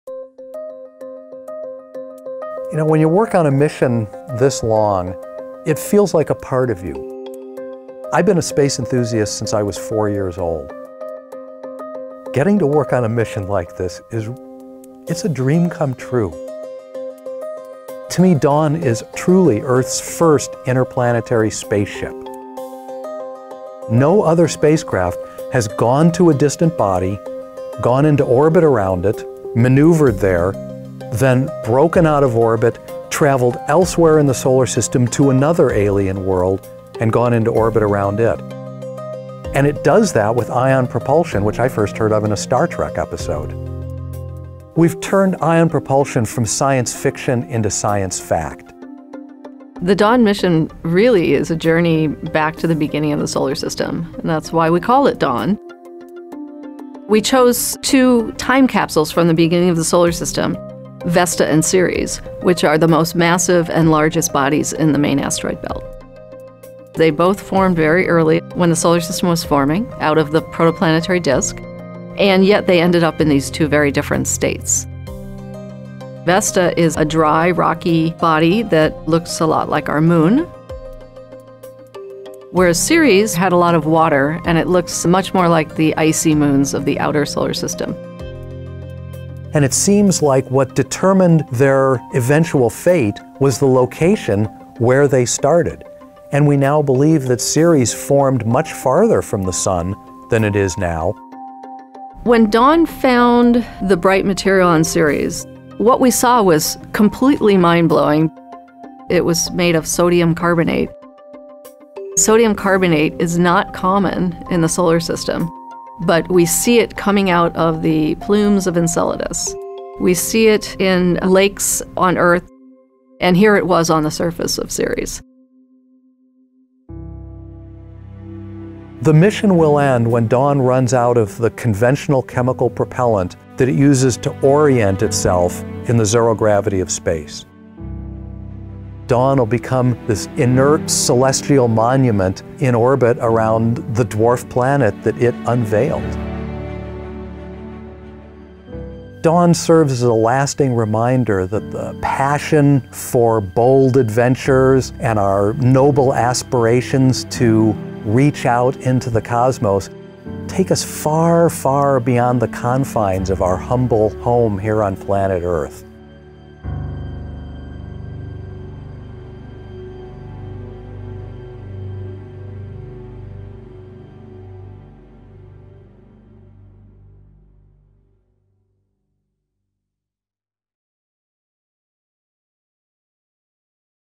Dusk for Dawn: NASA Mission to the Asteroid Belt
NASA’s Dawn spacecraft turned science fiction into science fact by using ion propulsion to explore the two largest bodies in the main asteroid belt, Vesta and Ceres. The mission will end this fall, when the spacecraft runs out of hydrazine, which keeps it oriented and in communication with Earth.